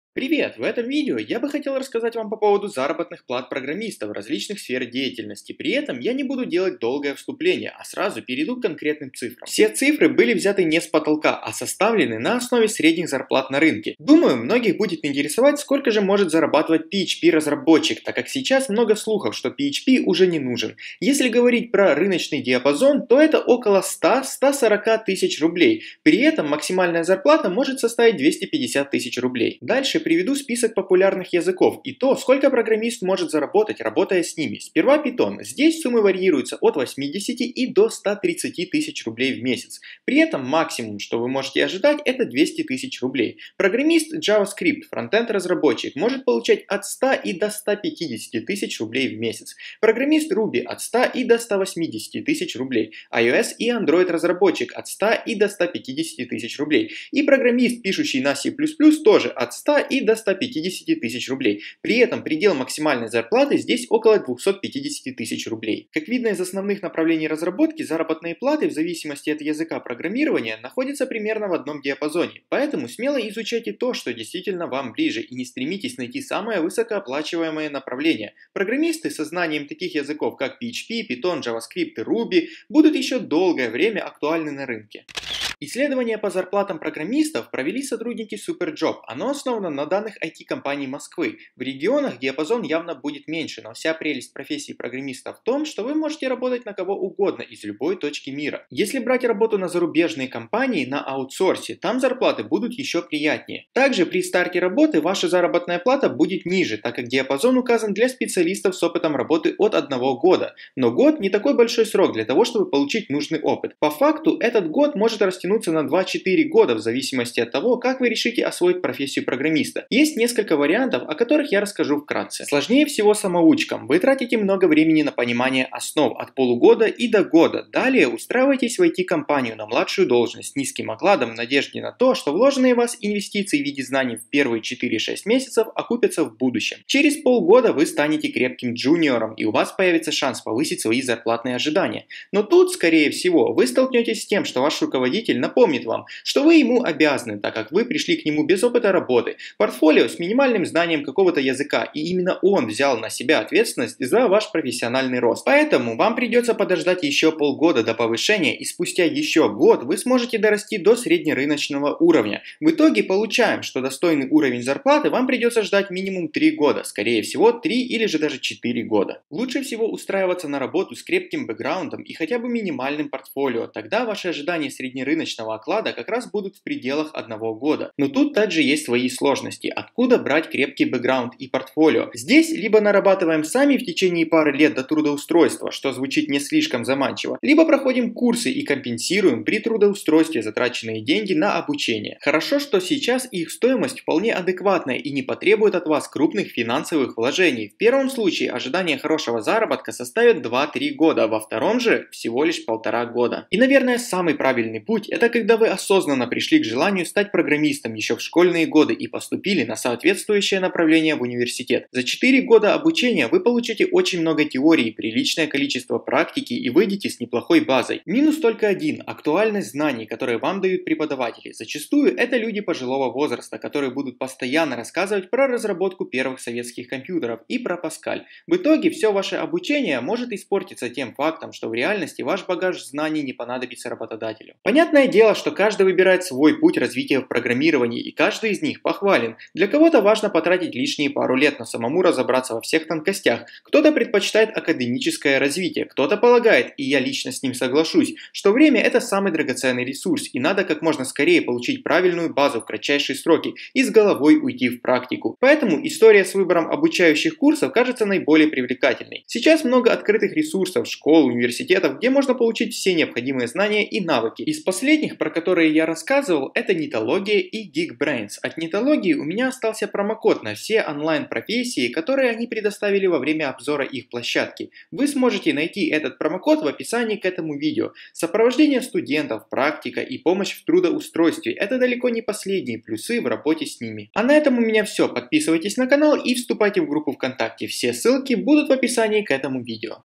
Сколько зарабатывают программисты?
*действует на профессии «Веб-разработчик», «Frontend-разработчик», «Python-разработчик», «Fullstack дизайнер»

В этом видео я расскажу вам про средние зарплаты программистов. Я назову конкретные суммы, которые составлены на основании средних зарплат по Москве. Кроме того, я расскажу вам каким образом можно начать зарабатывать такие зарплаты.

* Кошельки WebMoney:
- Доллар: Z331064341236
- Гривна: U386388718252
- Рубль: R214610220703